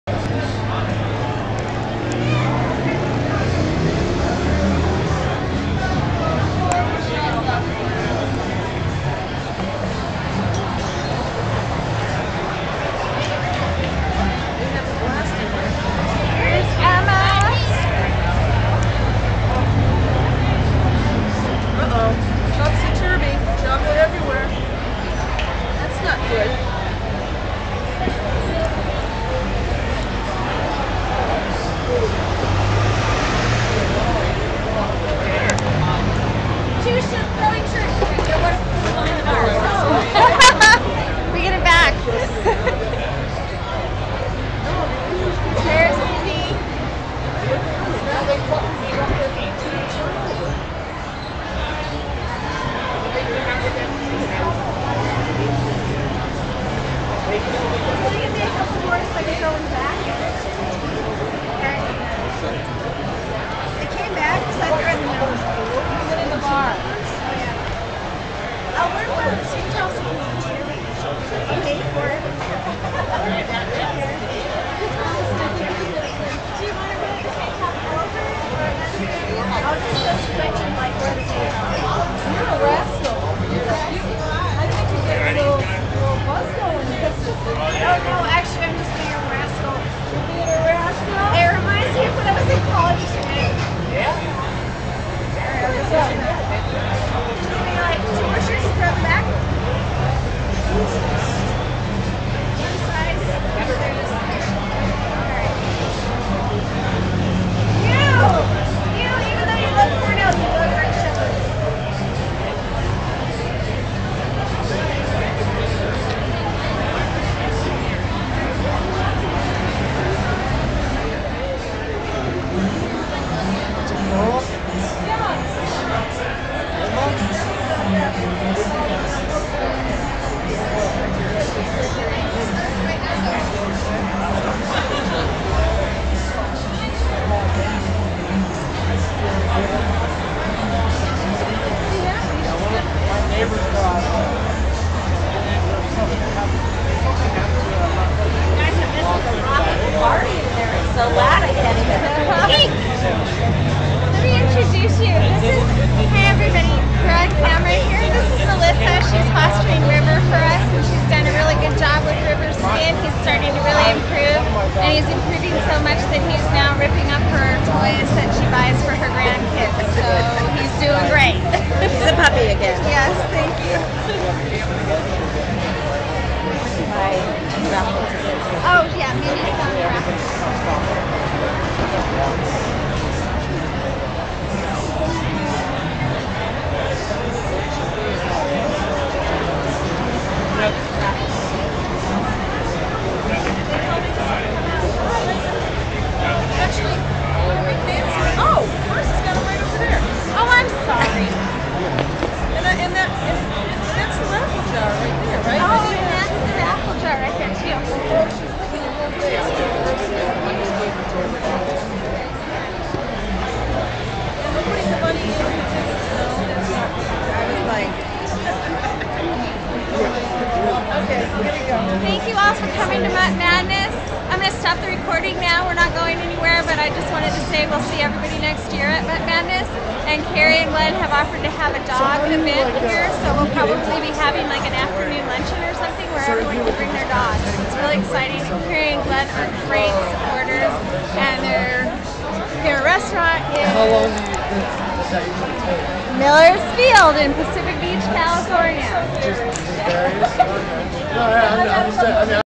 Mutt Madness at Millers Field - SCGSR Fundraiser 2010!
Millers Field in Pacific Beach, California, generous supporters of SCGSR, host Mutt Madness 2010!